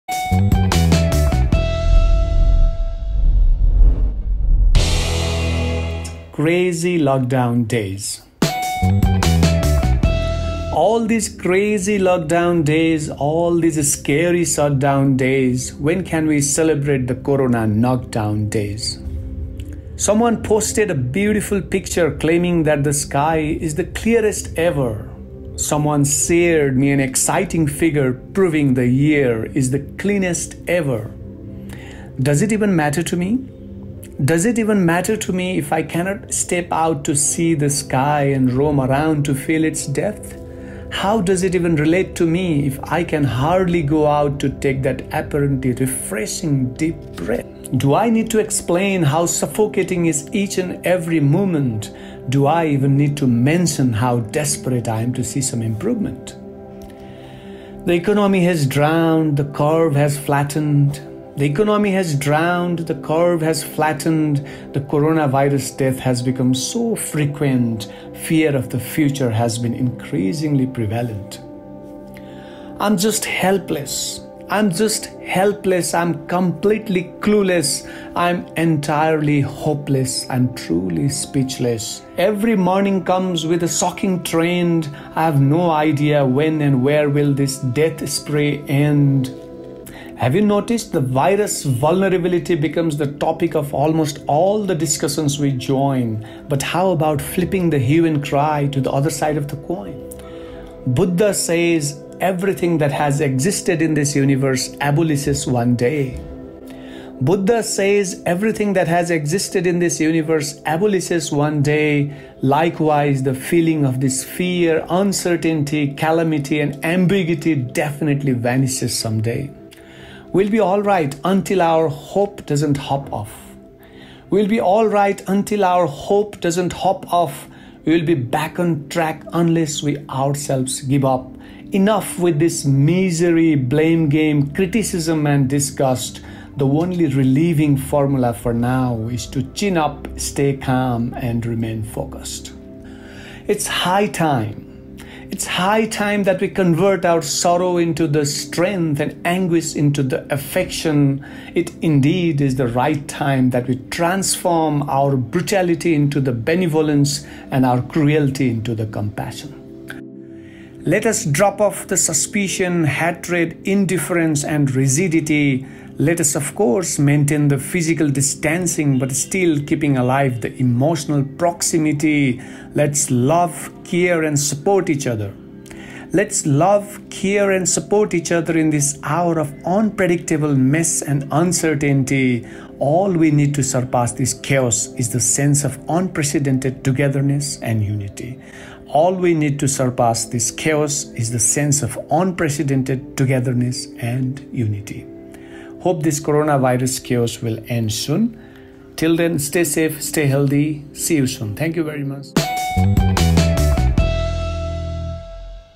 Crazy Lockdown Days | Visual Poem | Mahesh Babu Timalsina
Poem Title: Crazy Lockdown Days
Written & Recited by: Mahesh Babu Timalsina
Video Edited by: Rabi Lama (Kangaroo Creations)